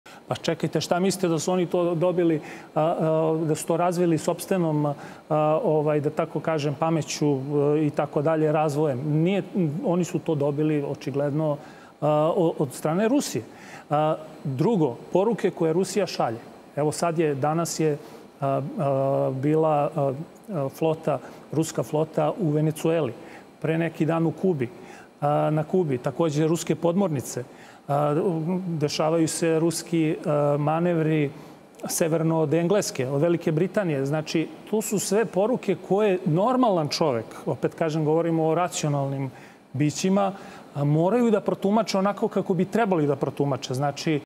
Rukski manevri severno od Engleske, podmornice kod Kube... - Rusija upozorava, zapad posmatra
Gosti: Prof Dragana Mitrović - Direktorka Instituta za azijske studije i redovni profesor Fakulteta političkih nauka Univerziteta u Beogradu
Danilo Tvrdišić - Novinar
Ranko Gojković - Publicista